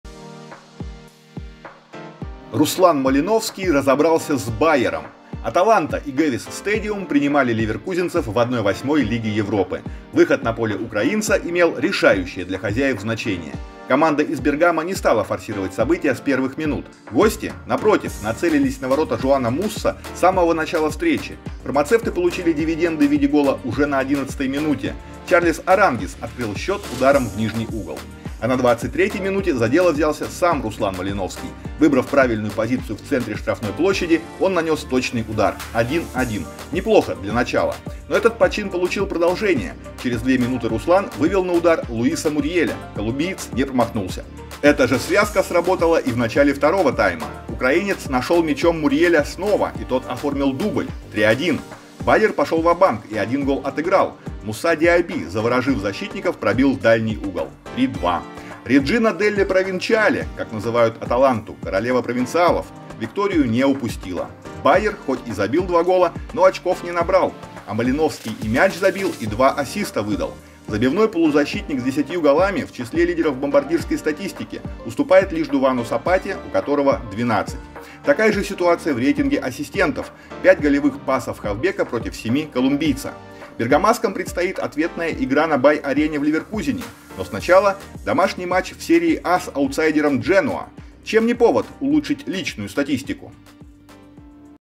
Малиновский герой Аталанты
«Аталанта» обыграла «Байер». Малиновский забил гол и отдал две голевые передачи